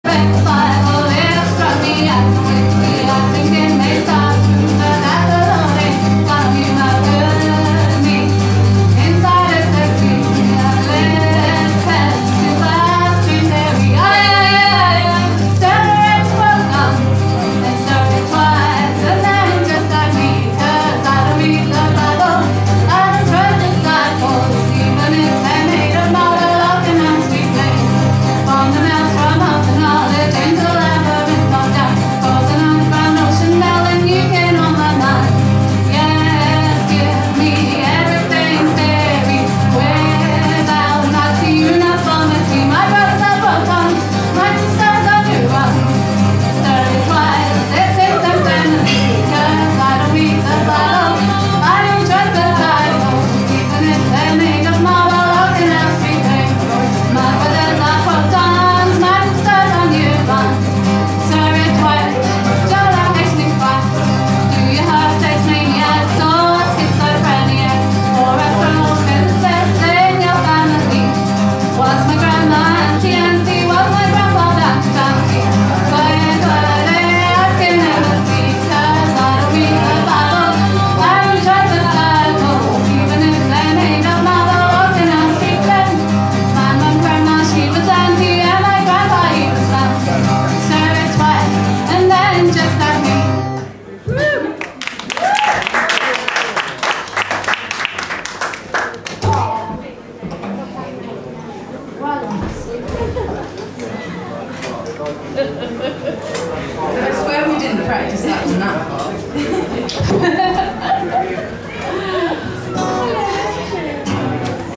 Daisy and a Stabiliser
Debut performance of Daisy and a Stabiliser at Norwich Underground. (Sorry the sound is a little distorted)